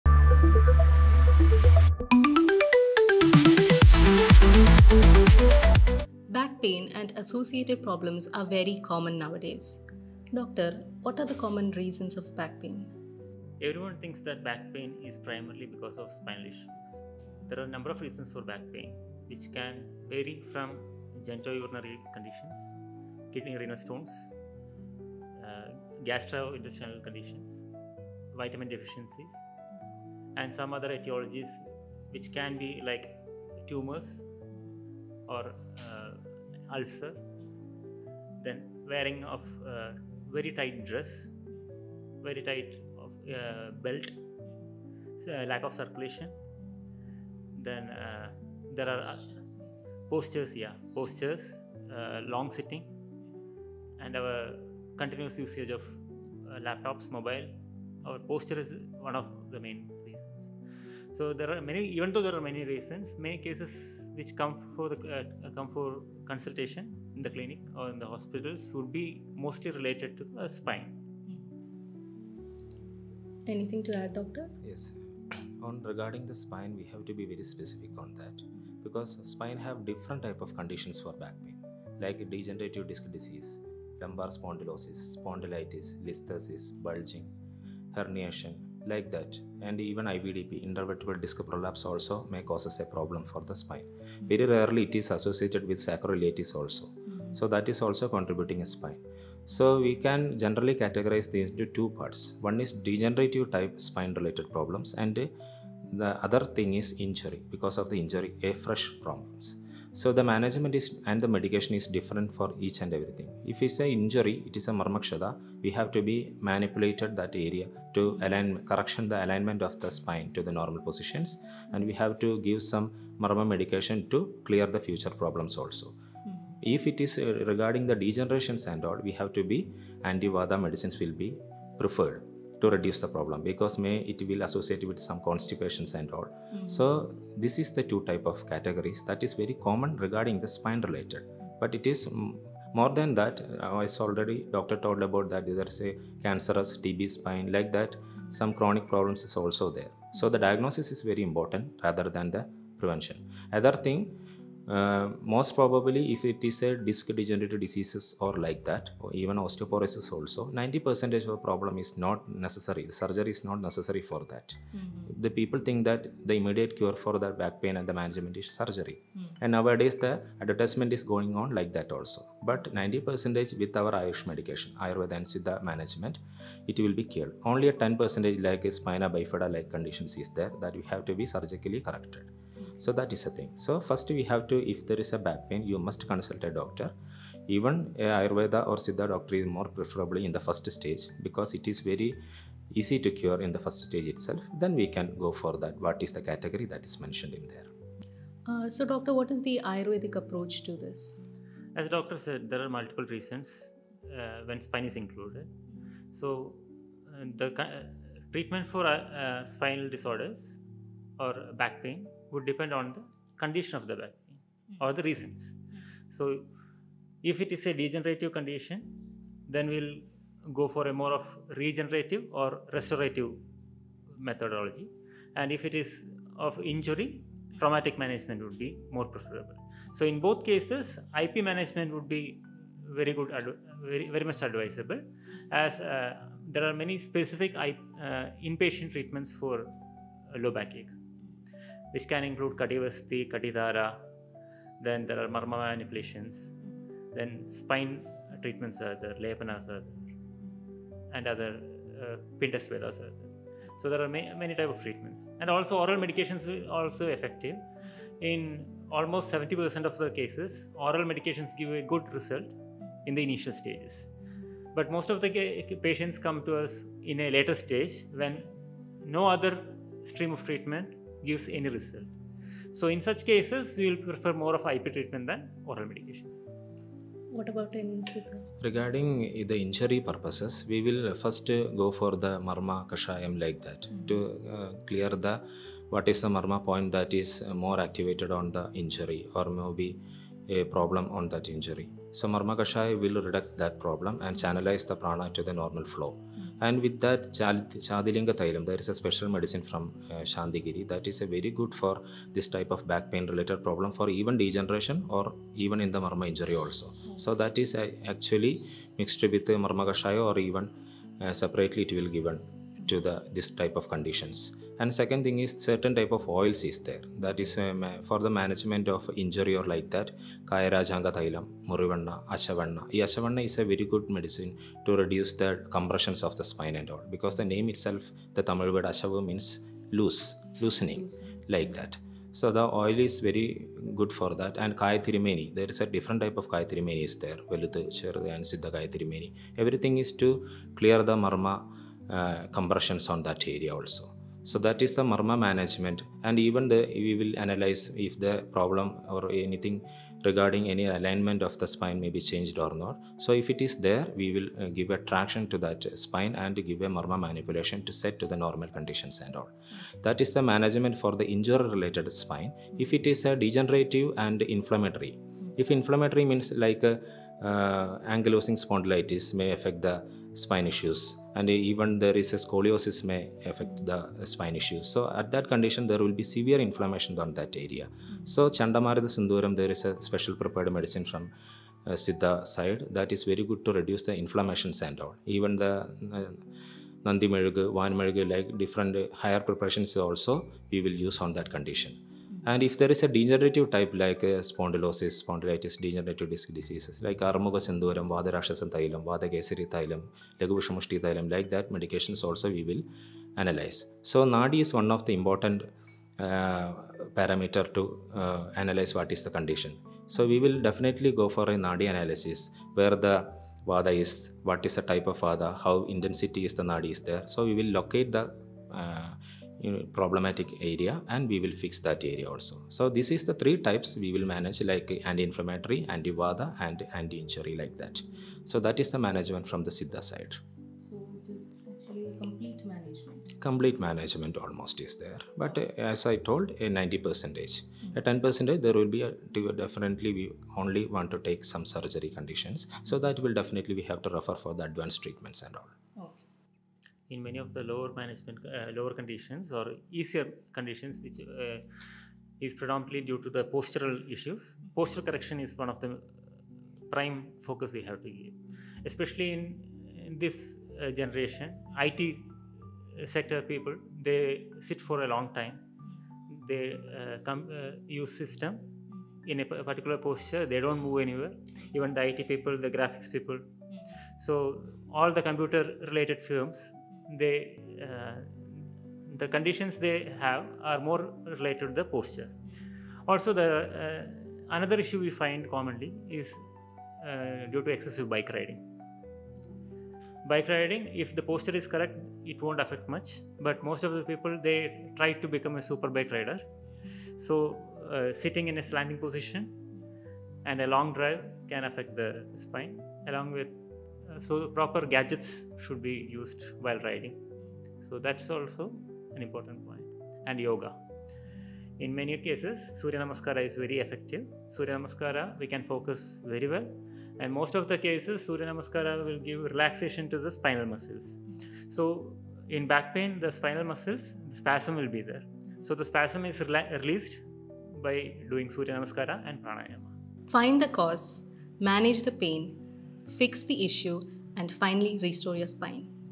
Doctor Podcast Ep15 | Back Pain
Podcast witha  Dr. Vivek V BAMS
Additional Medical Officer (Ayurveda)
Santhigiri Ayurveda Medical College Hospital and
Dr. Shabel. P.V BSMS
 Medical Officer (Siddha)
Santhigiri Medical Services